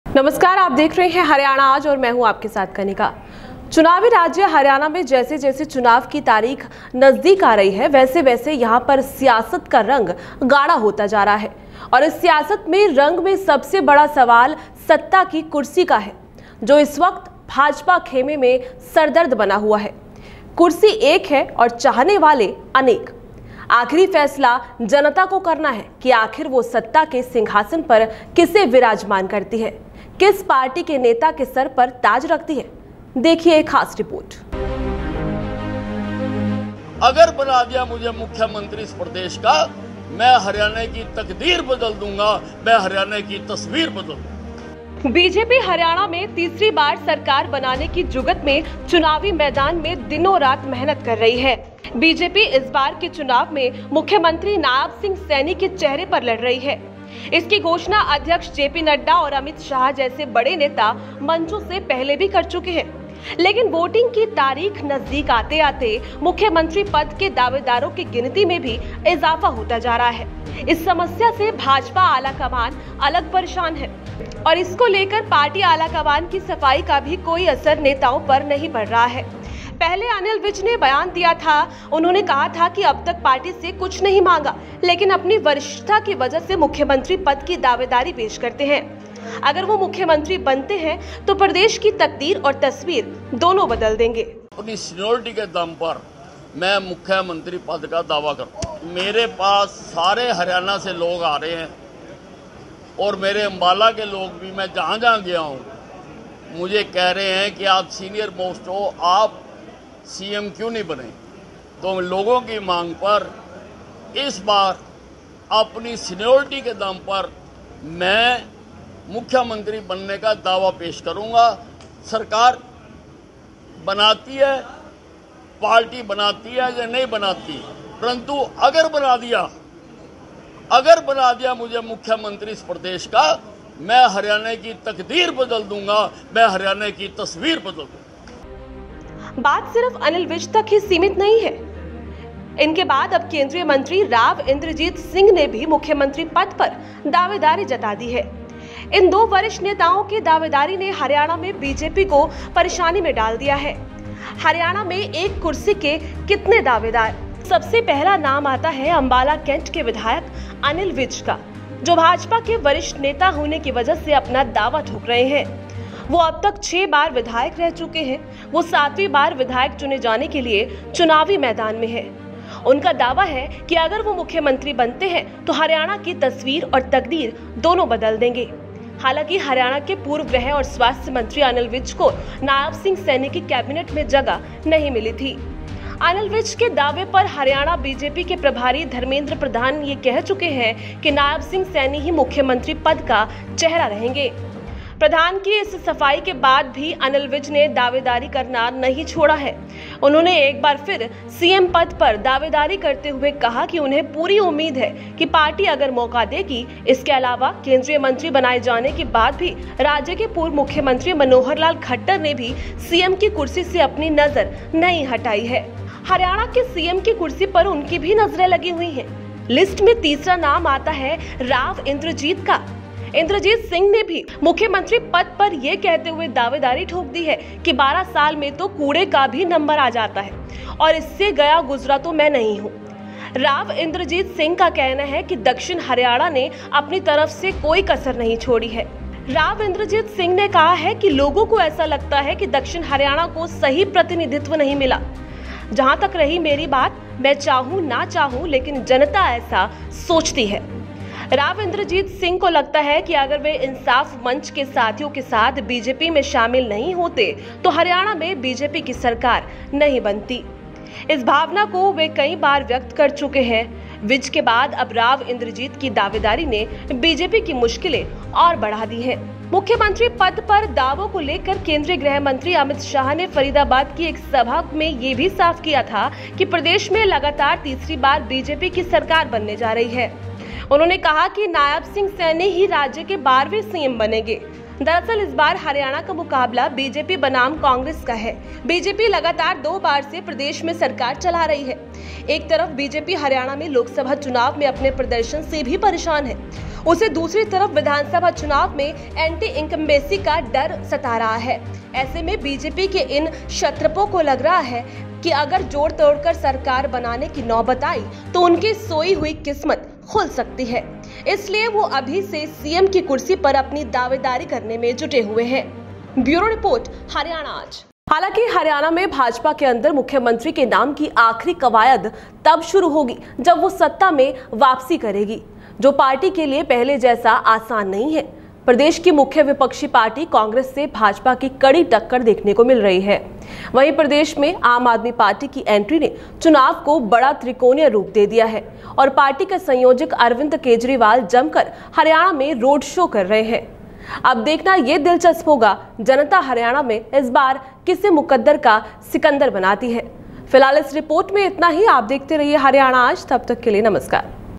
BJP में सीएम कुर्सी को लेकर घमासान, अपने ही करेंगे खेला! Haryana Election News
भाजपा में सीएम कुर्सी को लेकर घमासान, अपने ही करेंगे खेला! Haryana Election News